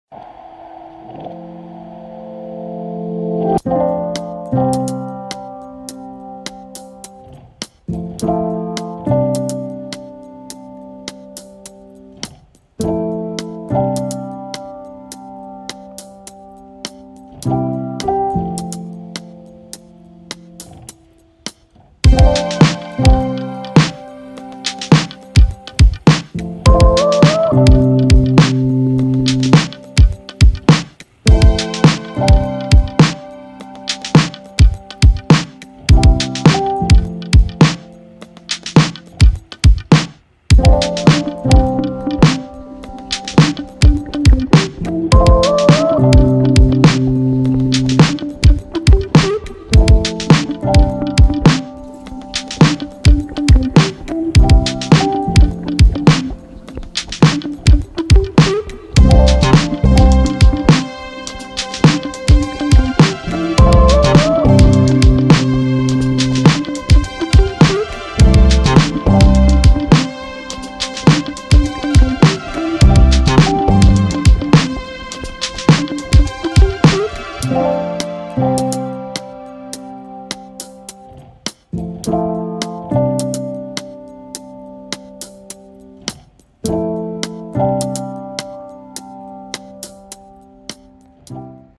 [Spinning effect] ATS ring Spinner V1 stainless steel.
Big list of fidget spinner by video
Name:  Ats mini
Brand: all things spinning
Material: Stainless steel
Weight: 45 gram
Price: new 140$ 2nd market 80$ to 100$